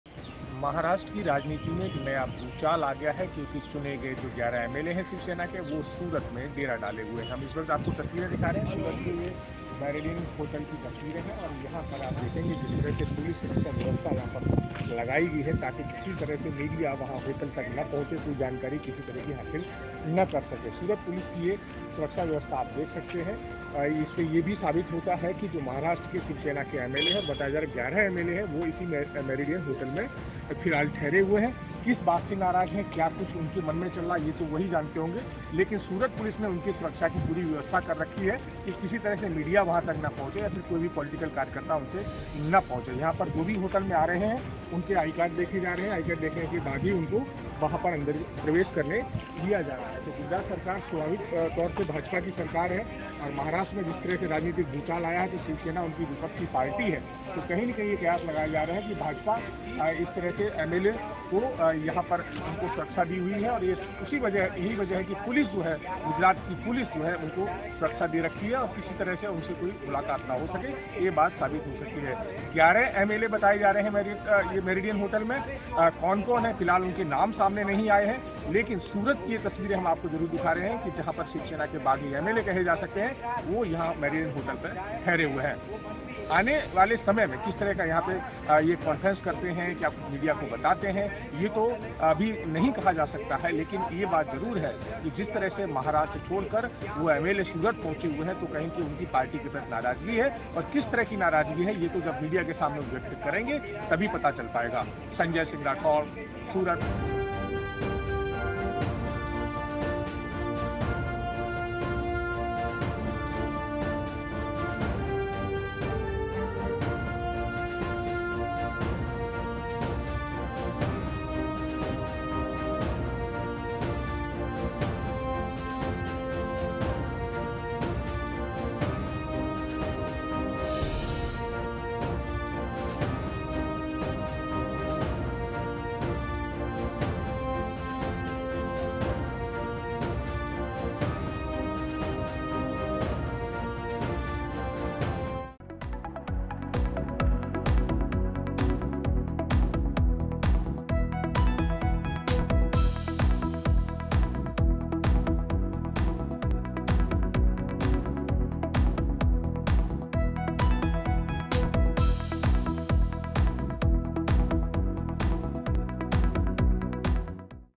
LIVE :Uddhav Thackeray | Eknath Shinde not Reachable |Shiv Sena| શરદ પવારે તમામ MLAની ઈમરજન્સી બેઠક
'મહા'વિદ્રોહ: શરદ પવારે તમામ ધારાસભ્યોની ઈમરજન્સી બેઠક બોલાવી, ગુજરાતમાં ઘડાઈ રહ્યો છે મોટો પ્લાન!

Maharashtra Politics : મહારાષ્ટ્રમાં વિધાન પરિષદની ચૂંટણી પછી મંત્રી એકનાથ શિંદેની આગેવાનીમાં શિવસેનાના 30 ધારાસભ્યો ગુજરાતની હોટલમાં રોકાયો હોવાનું સૂત્રો પાસેથી જાણવા મળ્યું છે. સુરતની લા મેરિડિયન ખાનગી હોટલમાં રોકાયા છે.